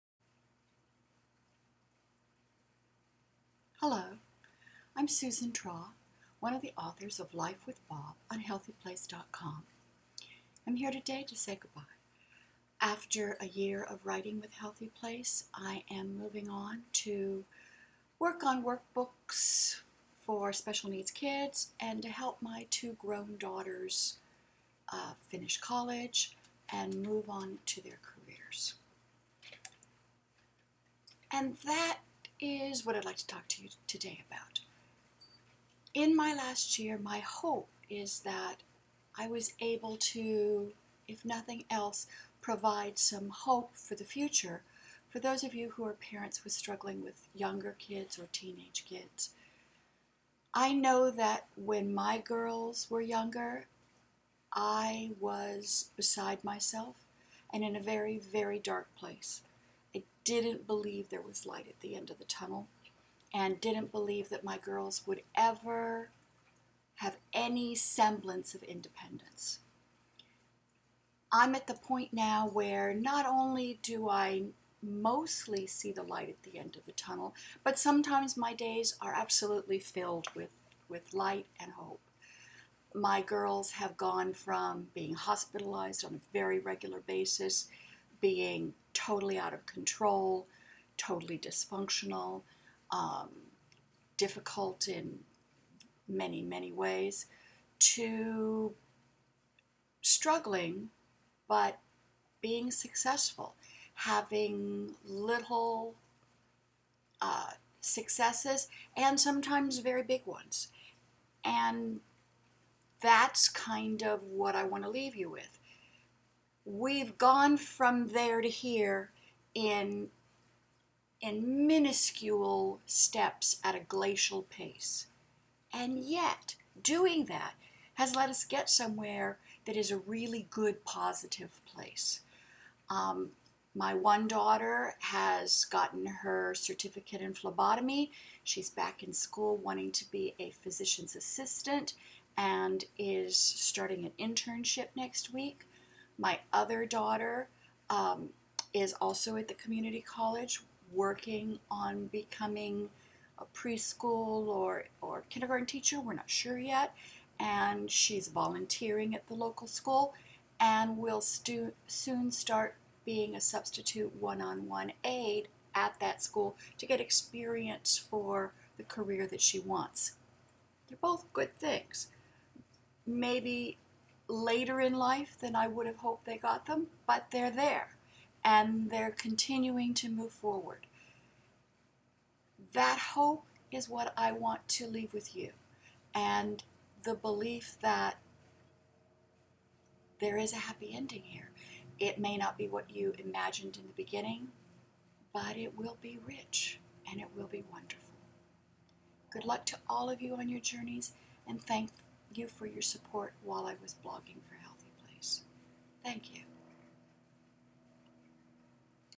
Good bye and Good Luck from Susan Traugh
Susan Traugh of "Life with Bob" says good-bye and good-luck to all of her followers and fellow bloggers at HealthyPlace.com